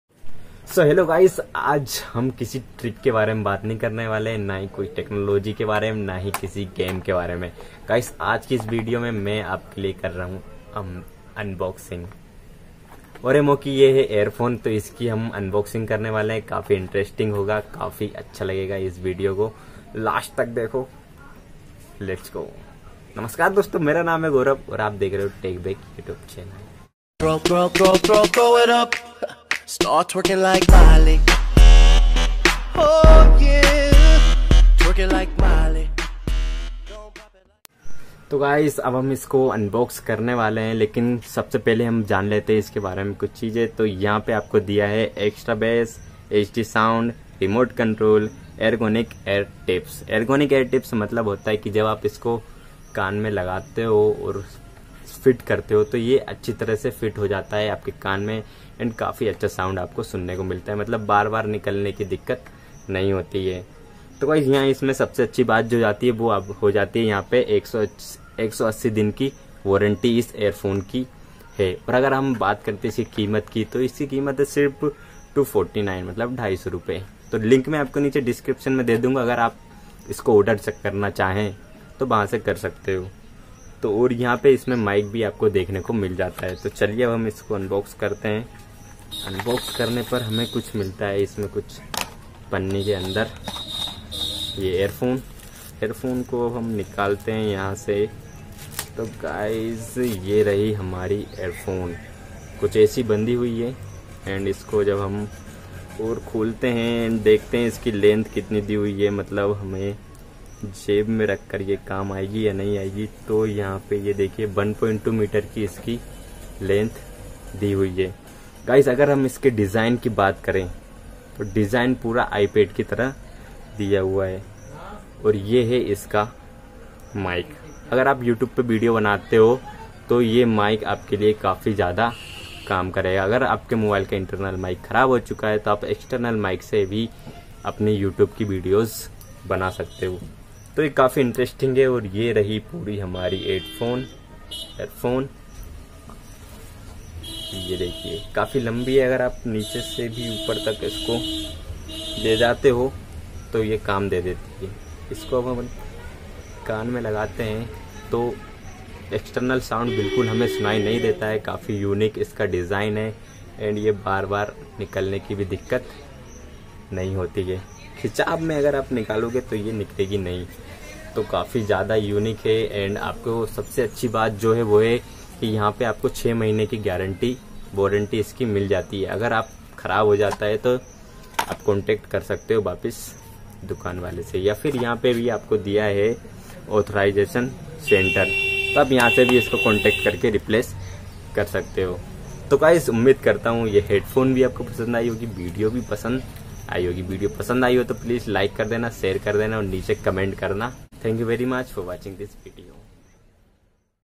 Oraimo Earphone Unboxing & Review 🔥🔥
Hello dosto,
Aaj Mene apke liye bahut hi achhi earphone ki Unboxing ki hai agar apko ye pasand aati hai to aap isko order karo.
Lekin mere hisaw se to kabhi achhi earphone hai apko Jaroor Leni chiye agar apko earphone ki jaroorat hai .
Low budget me kafi achhi earphone apko mil jaygi.
Ummid karta hu apko ye video pasand ayegi